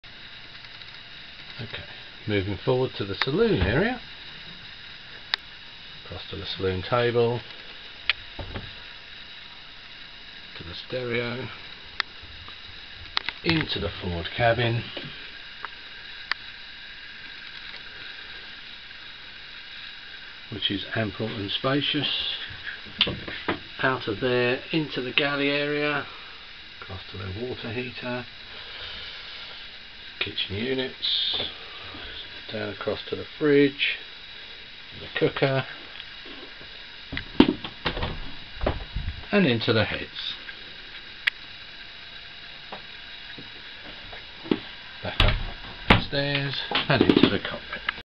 Barbary Ketch Motor Sailer - Boatshed.com - Boat Ref#151627
Barbary Ketch Motor Sailer for sale with Boatshed Suffolk Photos and video taken by Boatshed Suffolk